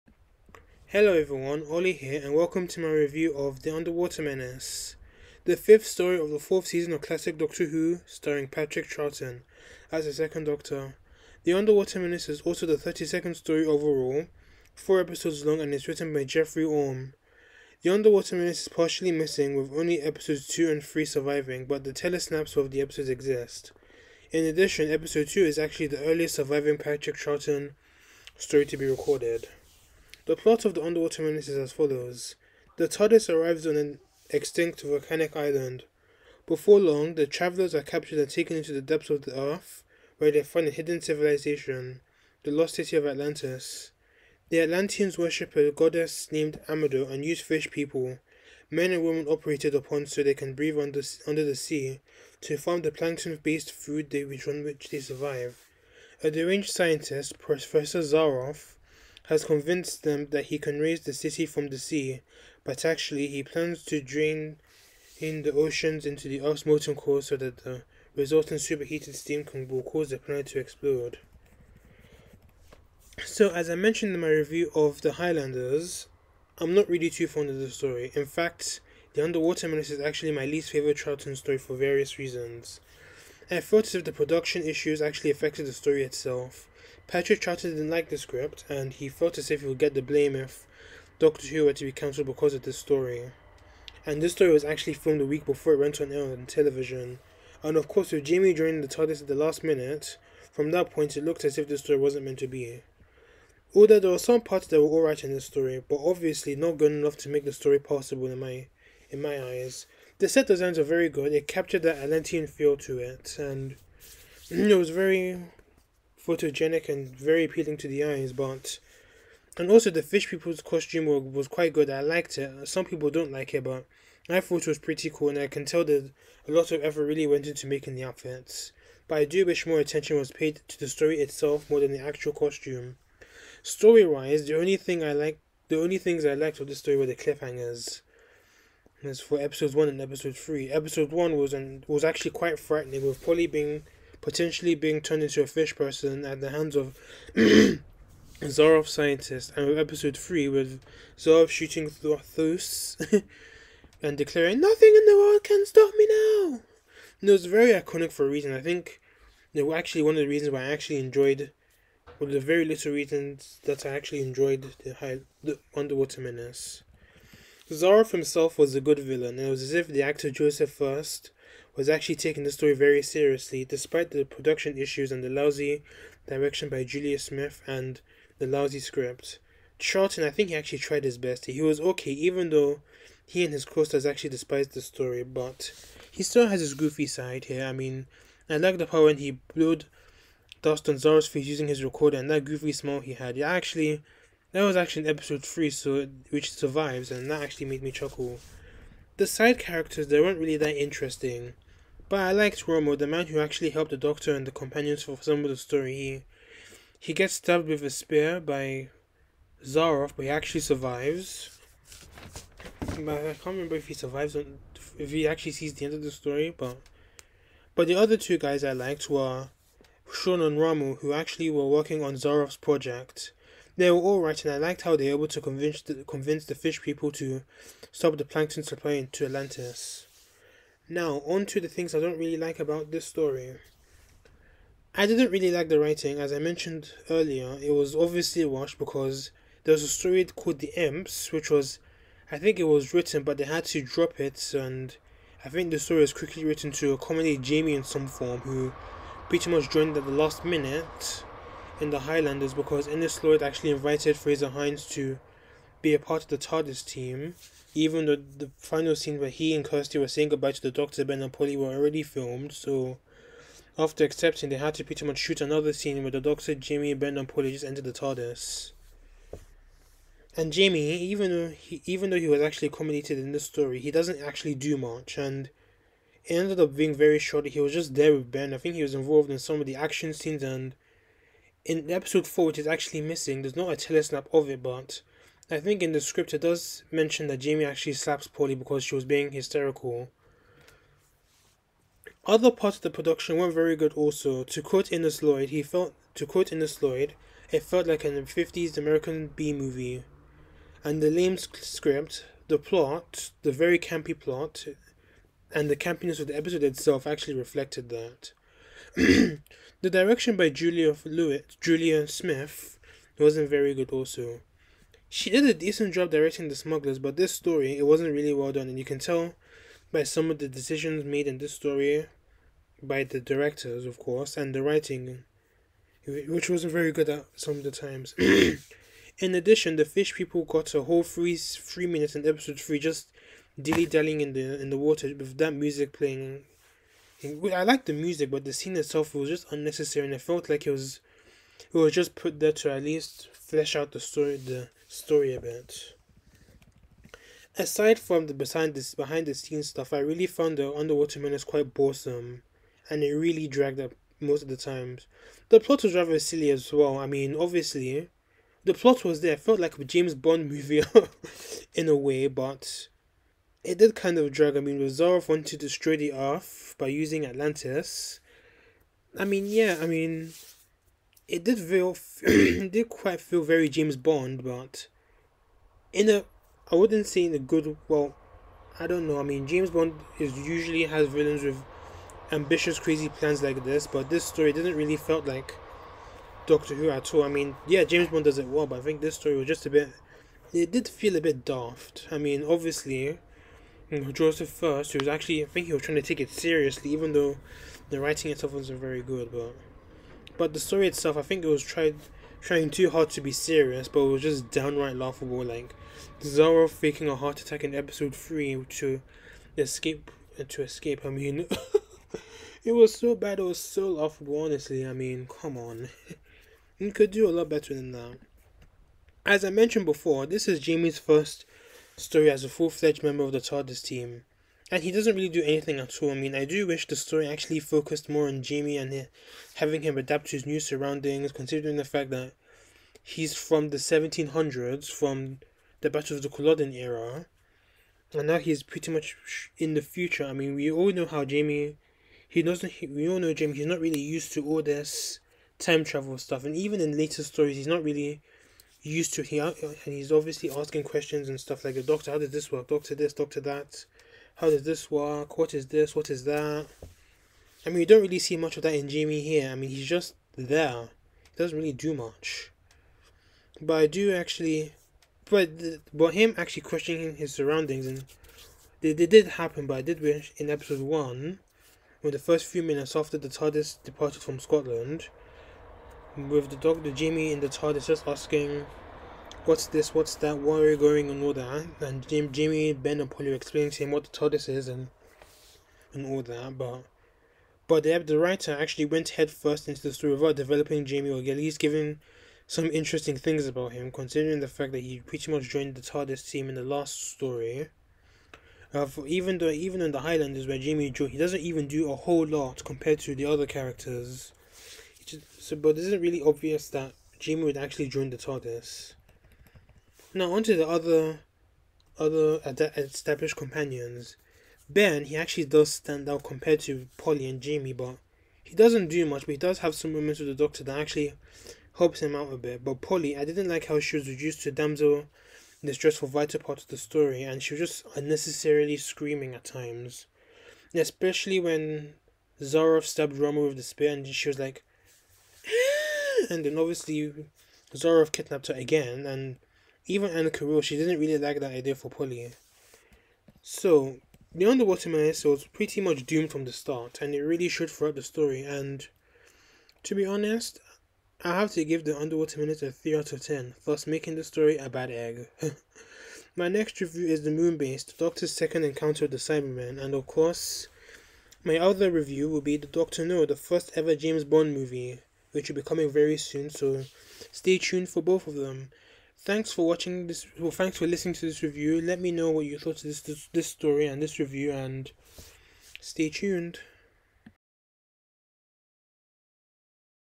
Classic Doctor Who Review S4/E5 - "The Underwater Menace" (Audio)
Hi everyone, this is the fifth story of Season 4 and the Fifteenth missing episode, "The Underwater Menace".